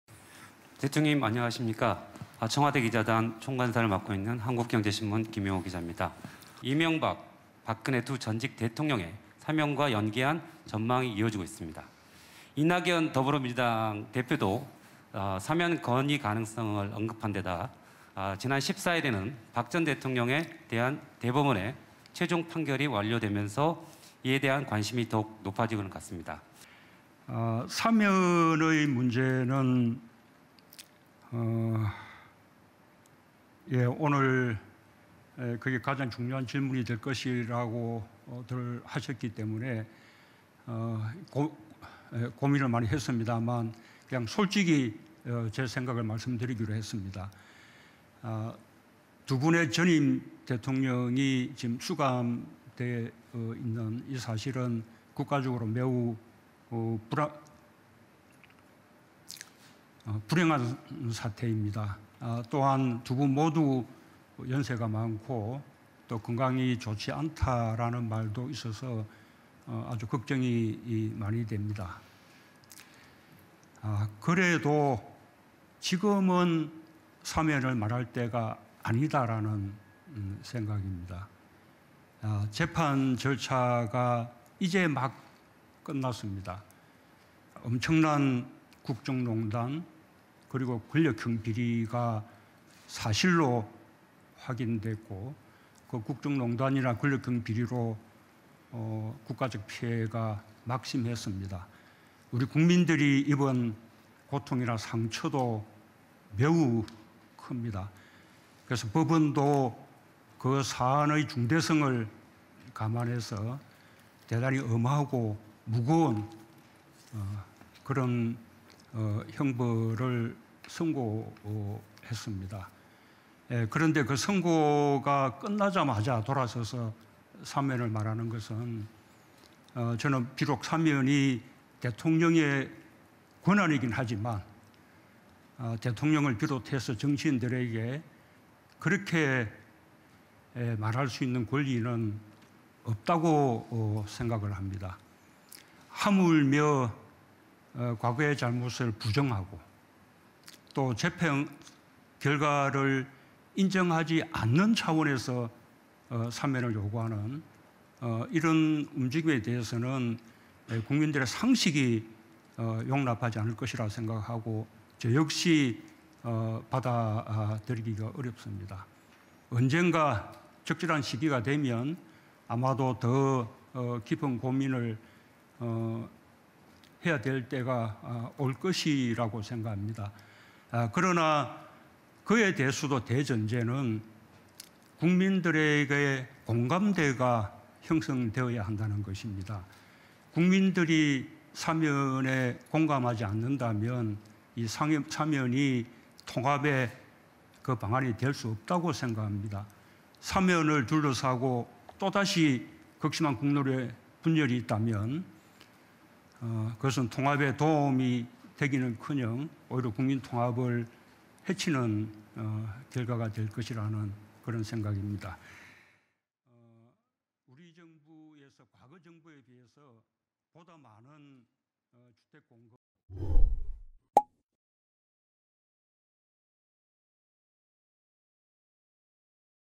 문대통령 신년 기자회견…"사면, 지금은 말할 때 아니다" / 연합뉴스 (Yonhapnews)
(서울=연합뉴스) 문재인 대통령은 18일 이명박·박근혜 전 대통령 사면 문제와 관련해 "지금은 사면을 말할 때가 아니다"라고 말했습니다.
    문 대통령은 이날 청와대 춘추관에서 열린 신년 기자회견에서 이에 대한 질문을 받자 "고민을 많이 했지만 솔직히 제 생각을 말씀드리기로 했다"며 이같이 밝혔습니다.
    문 대통령은 "두 전임 대통령이 수감된 사실은 국가적으로 매우 불행한 사태이며 두 분 모두 연세가 많고 건강이 좋지 않다는 말이 있어 걱정이 많이 된다"면서도 "재판 절차가 이제 막 끝났다. 엄청난 국정농단과 권력형 비리가 사실로 확인됐고 국가적 폐해가 막심했고 국민이 입은 고통이나 상처도 매우 크다"고 지적했습니다.
    또 "선고가 끝나자마자 사면을 말하는 것은, 사면이 대통령의 권한이긴 하지만 대통령을 비롯한 정치인들에게 그런 권리는 없다고 생각한다"고 거듭 부정적 견해를 밝혔습니다.
    이어 "하물며 과거의 잘못을 부정하고, 또 재판 결과를 인정하지 않는 차원에서 사면을 요구하는 이런 움직임에 대해서는 국민들의 상식이 용납하지 않을 것"이라며 "저 역시 받아들이기 어렵다"라고 말했습니다. 영상으로 보시죠.